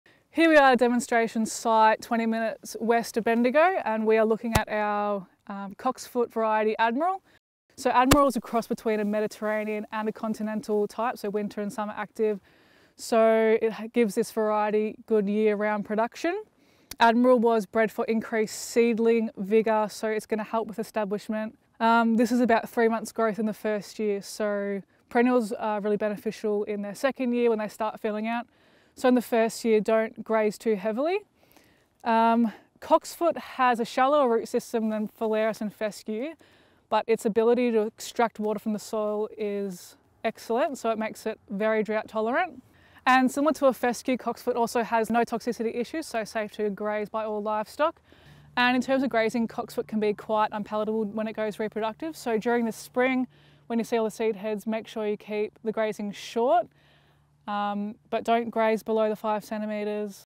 Admiral Cocksfoot
Admiral cocksfoot is a perennial grass suited to free- draining soils but also has good tolerance to acid and low fertility soils. Admiral is a cross between a continental and a Mediterranean cocksfoot which gives this cultivar more year-round production. Bred with increased seedling vigour ensuring a successful establishment, Admiral is paired well with sub clovers for a quality pasture stand. Suitable for sheep, beef, dairy and equine.